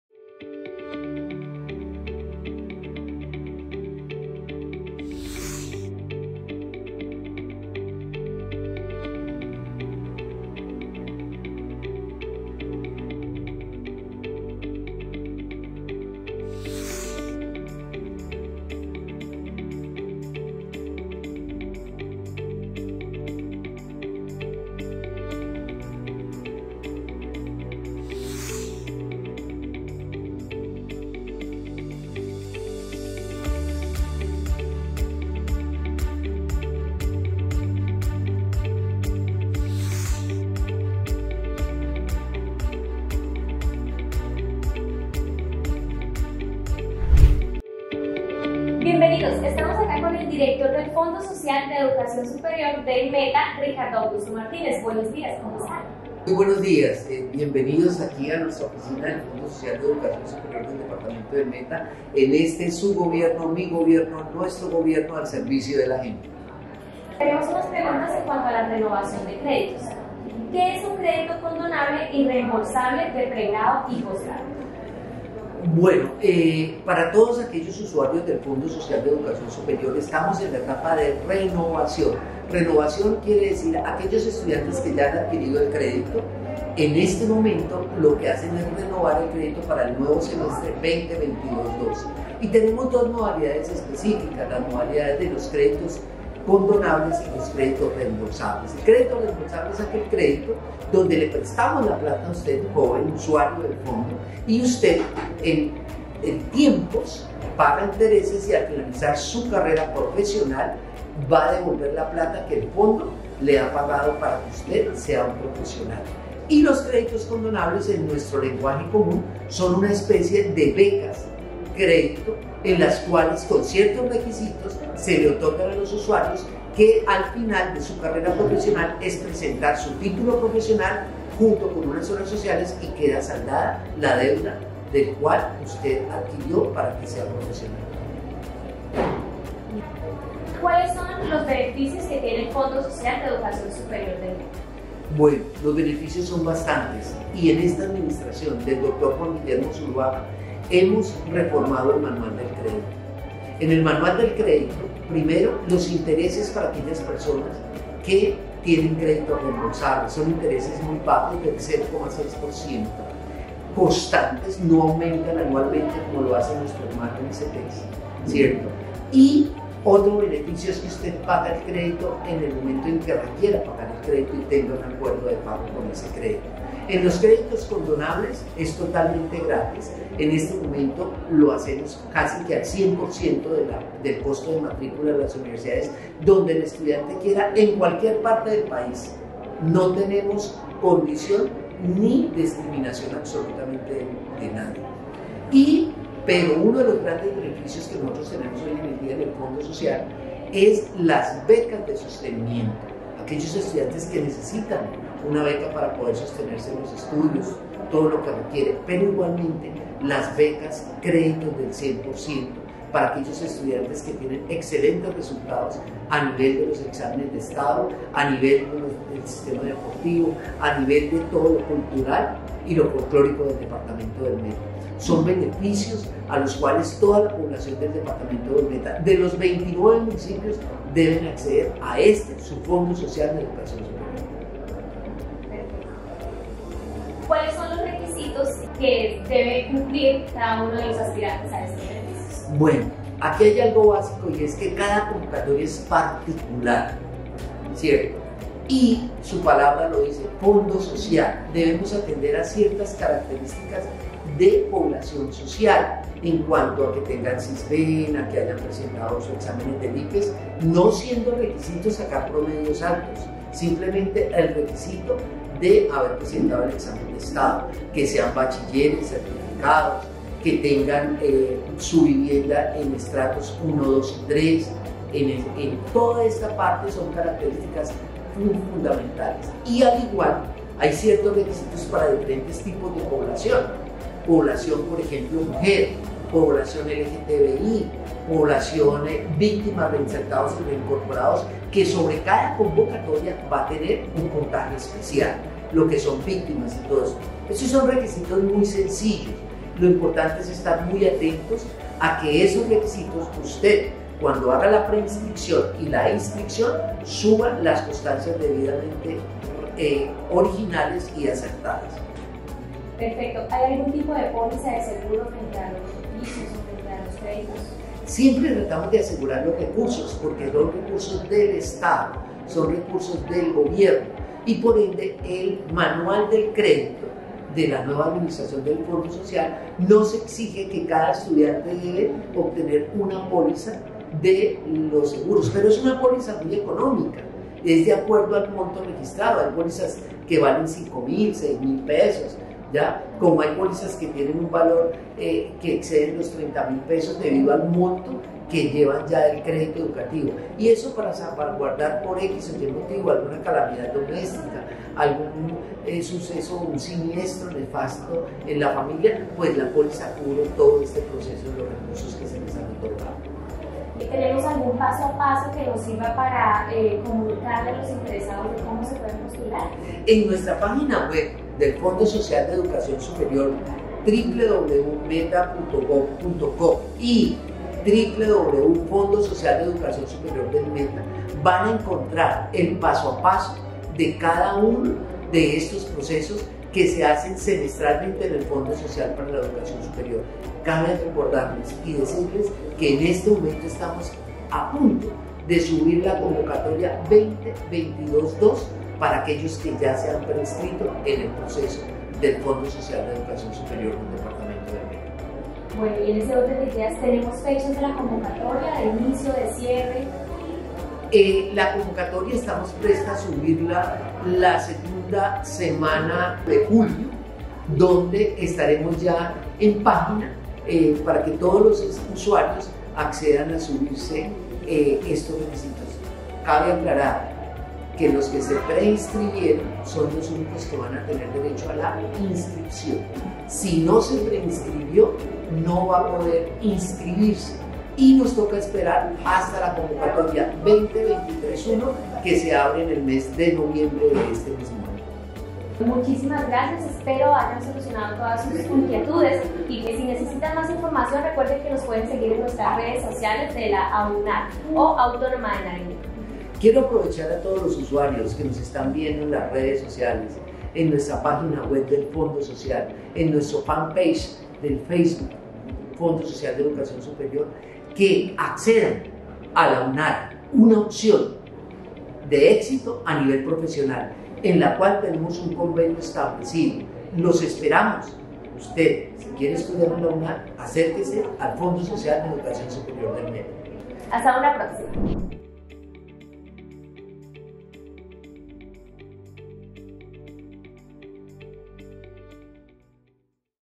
Renovación de créditos - Fondo Social de Educación Superior del Meta
Si eres beneficiario del Fondo Social de Educación Superior del Meta, esta información es para ti.
El director del FSES del Meta, el Dr. Ricardo Martínez, aclara nuestras dudas sobre la renovación de créditos y los diferentes beneficios que tienen para los estudiantes.
¡Tú lo sueñas, y la Autónoma lo hace realidad!